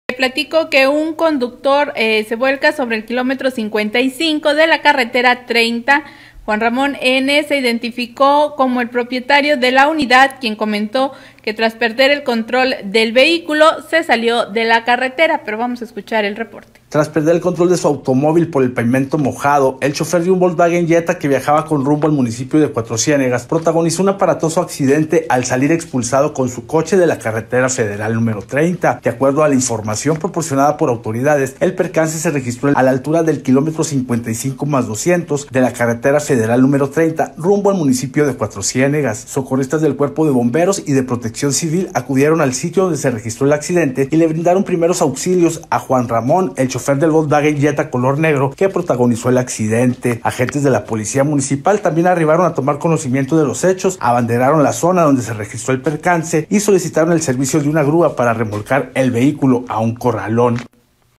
Conductor vuelca sobre el kilómetro 55 de la carretera 30.
Conductor vuelca sobre el kilómetro 55 de la carretera 30, Juan Ramón N se identificó como el propietario de la unidad, quien comentó que tras perder el control del vehículo salió de la carretera.